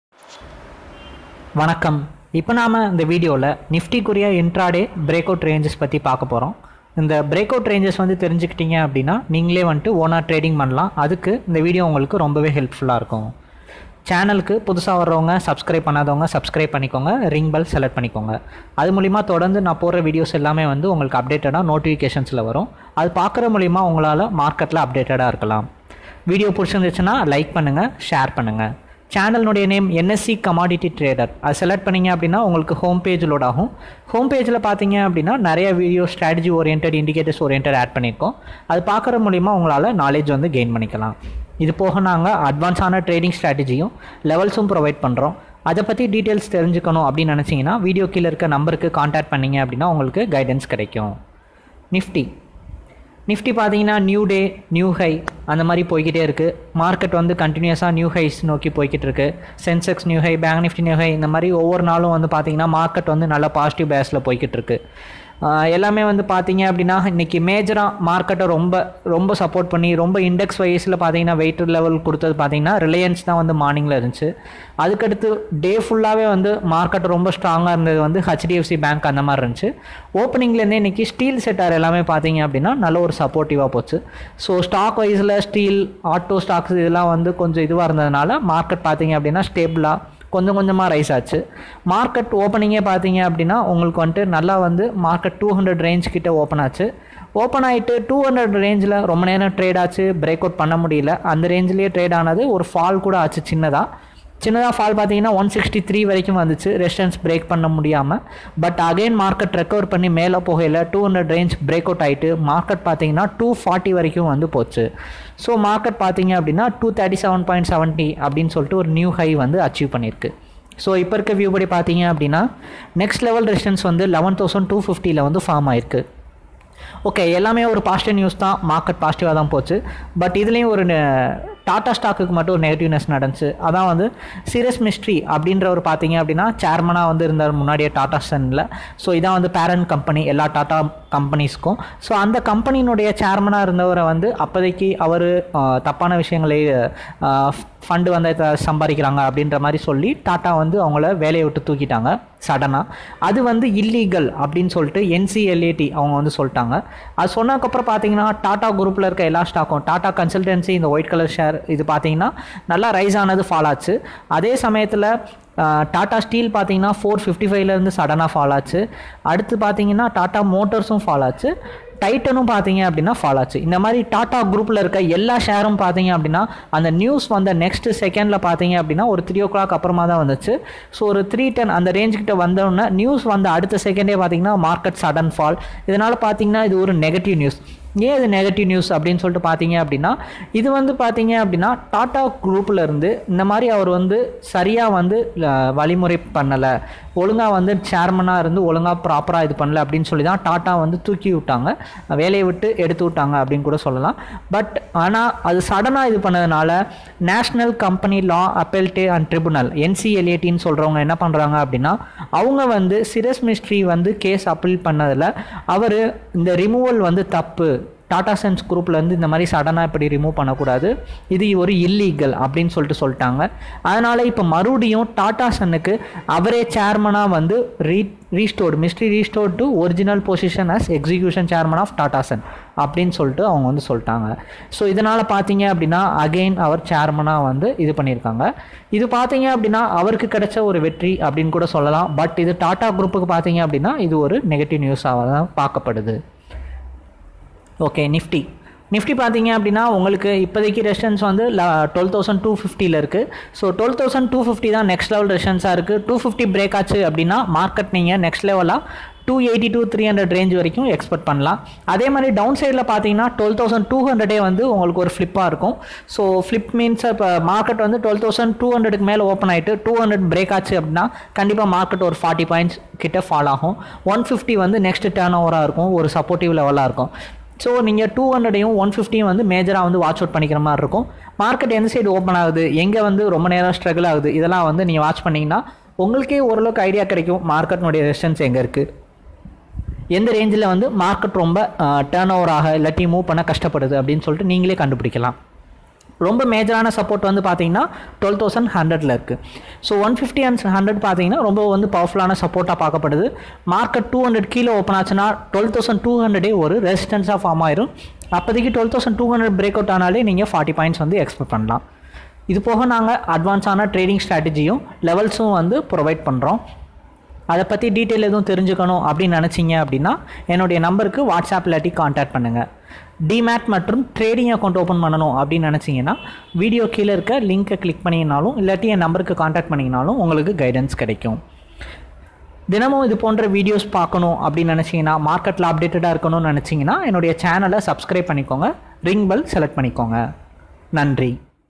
Nifty Trading Ideas | Equity Bullish | Intraday Breakout Range
Open account  by clicking LINK - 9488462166

This Video about NIFTY50 Breakout Range for Intraday Trading.

Nifty need further breakout at 12250 resistance
12150 act as support